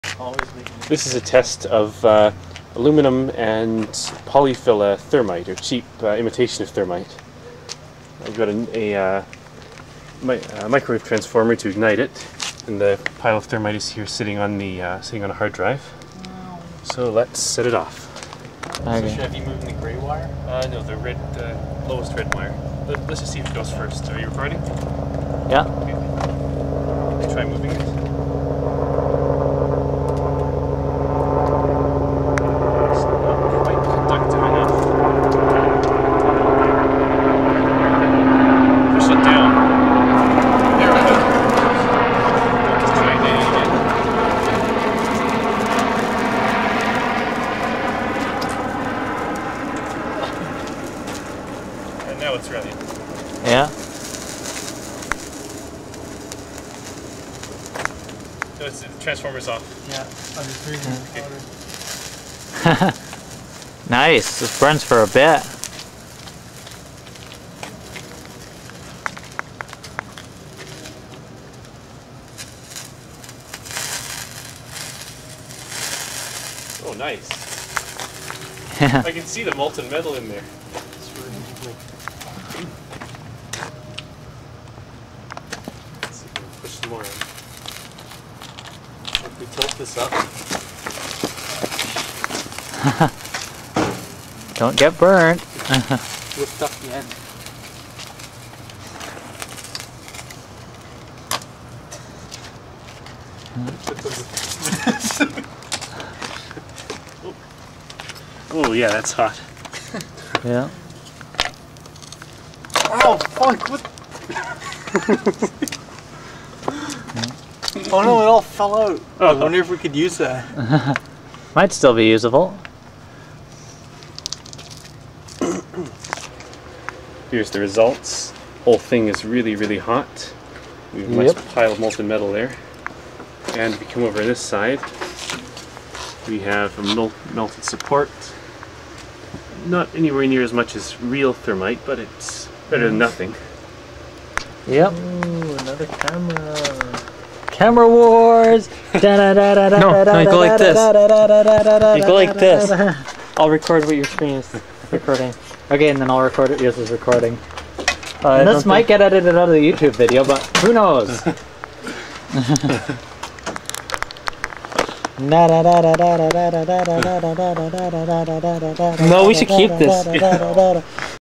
Imitation thermite using plaster and aluminum powder
We made some cheap imitation thermite using Polyfilla plaster and aluminum ground into powder with a bench grinder. We set it off using a microwave transformer to generate high heat initially. After the reaction started, it is self-sustaining.